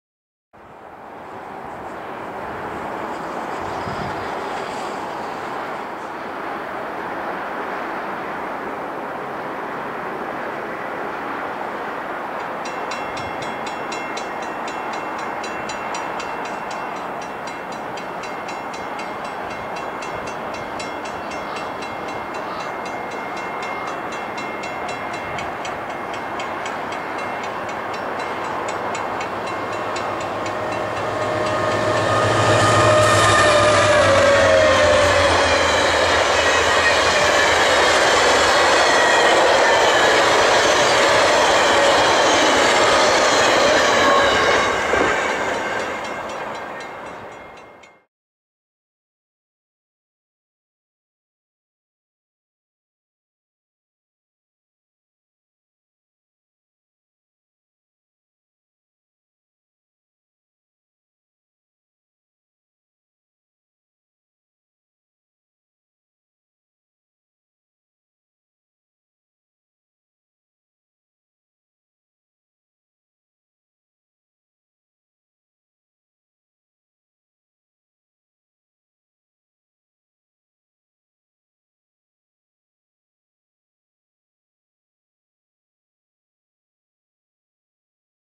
VIA #1 With Rebuilt Budd RDC's In Tow
November 26, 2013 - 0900. VIA #1 westbound at Acheson, Alberta with two refurbished Budd RDC's trailing the park car. Destined for Jasper where they will be tested on The Skeena (Jasper - Prince Rupert). Credit and thanks to Terry from Can-Passenger-Rail for the complete consist information.

VIA 6404 - F40PH-3
VIA 6435 - F40PH-3
8613 Baggage
8126
8144 Ex Amtrak
8511 Skyline
8401 Acadian Dining car
8329 Hearne Manor
8326 Franklin Manor
8314 Cameron Manor
8703 Banff Park
VIA 6219 - RDC - D/H Off at Jasper
VIA 6251 - RDC - D/H Off at Jasper